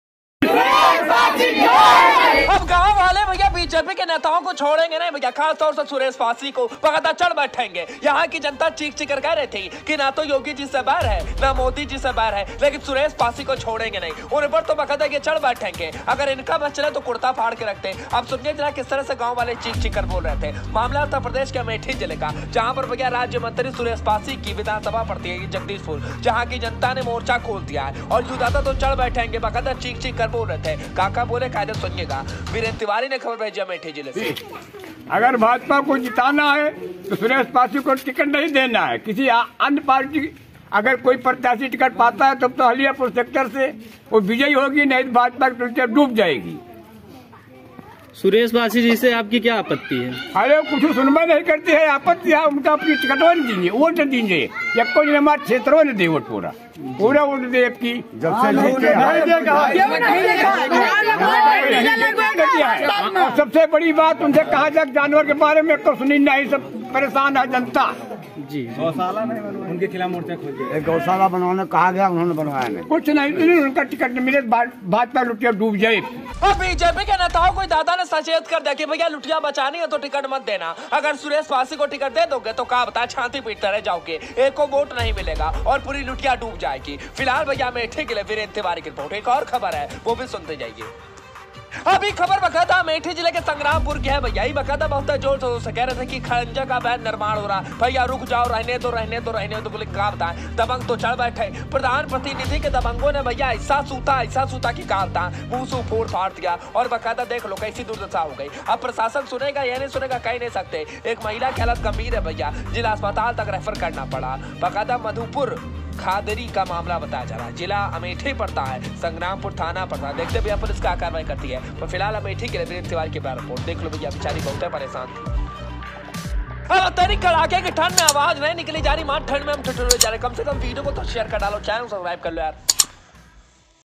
भाजपा नेताओं को घुसने नहीं देंगे गांव में दादा का गुस्सा ।। संग्रामपुर में हो गई महिला की हो गई..!
अमेठी- अवैध खण्डजा निर्माण का विरोध करना पीड़ित परिवार को पड़ा भारी,प्रधानपति ने दबंगों के साथ मिलकर पूरे परिवार की जमकर लाठी डंडे से की पिटाई,1 महिला गम्भीर हालत में जिला अस्पताल रेफर, संग्रामपुर थाना क्षेत्र के मधुपुर खदरी का मामला